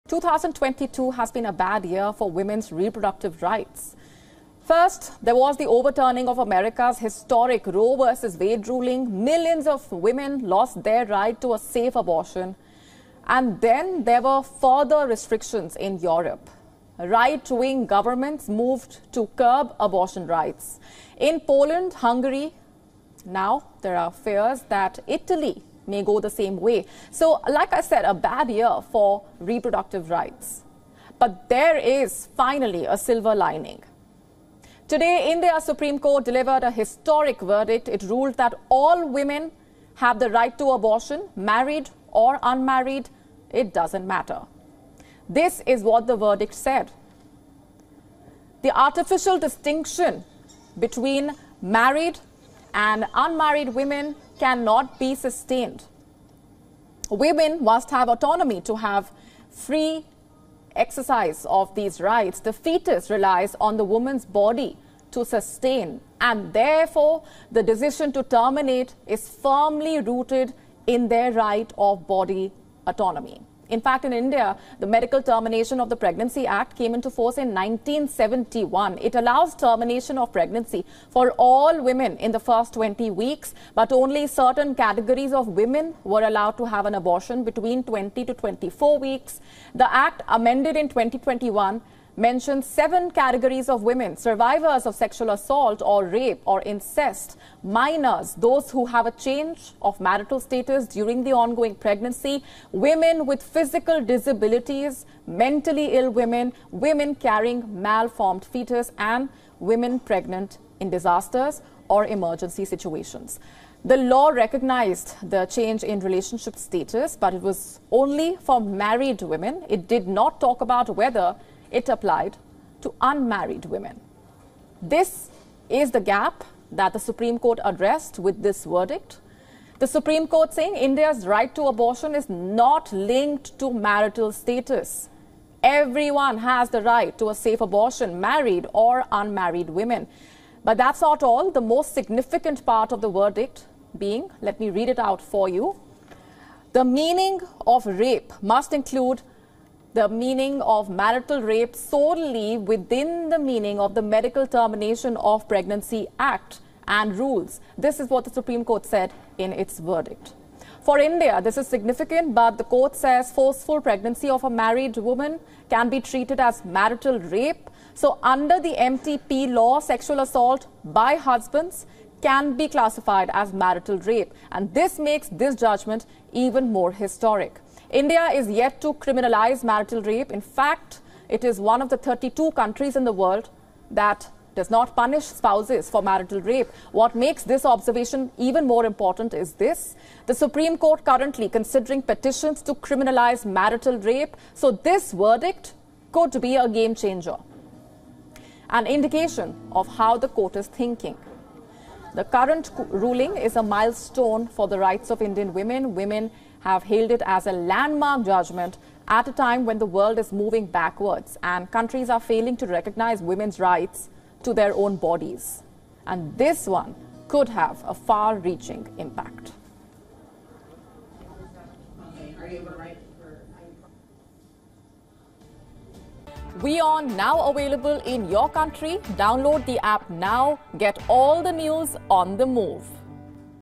WION Fineprint: India's Supreme Court delivers a landmark judgement on abortion rights
India's Supreme Court delivered a landmark judgement, which reiterated that all women have the right to get an abortion.